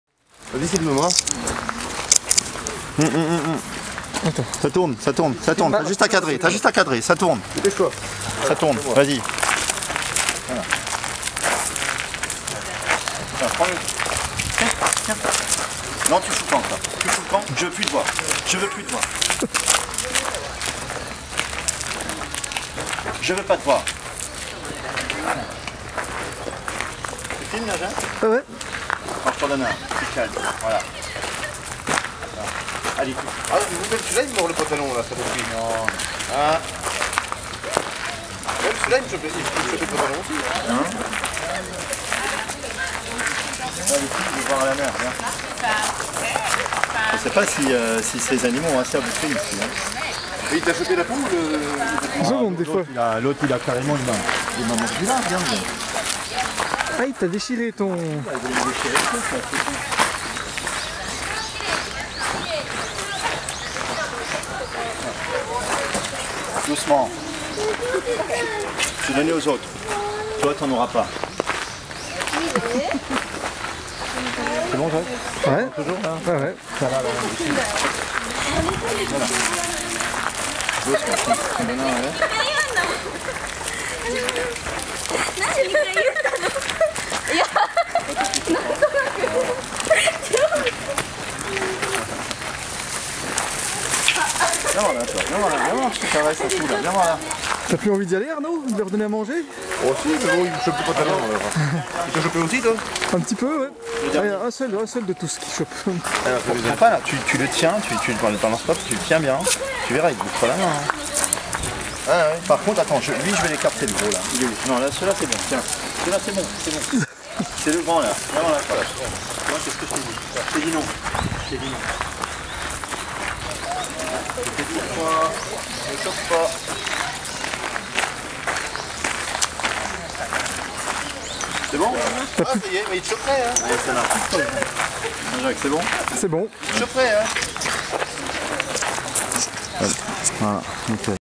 V 2013 04 23 Nara D4 3
ANNEE : 2013
MOIS : 04
JOUR : 23

LIEU : NARA
Parc du temple Kasuga-taisha

EVENEMENT :

PERSONNAGE(S) : Denis, Arnaud et les daims